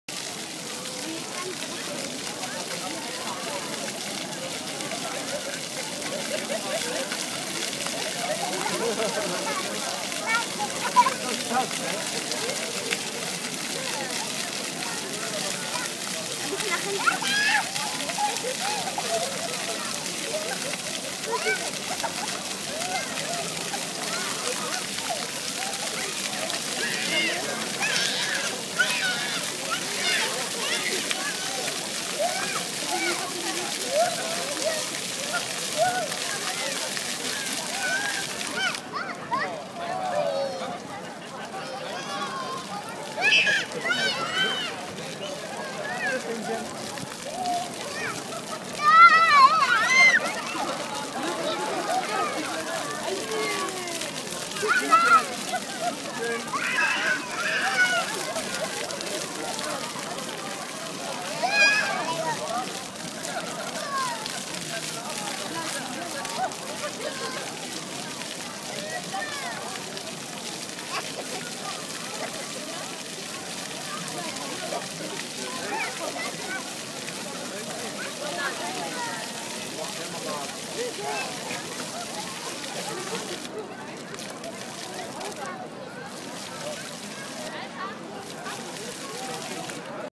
Dog eating water
Dog tries to catch fountain water shooting randomly from different places.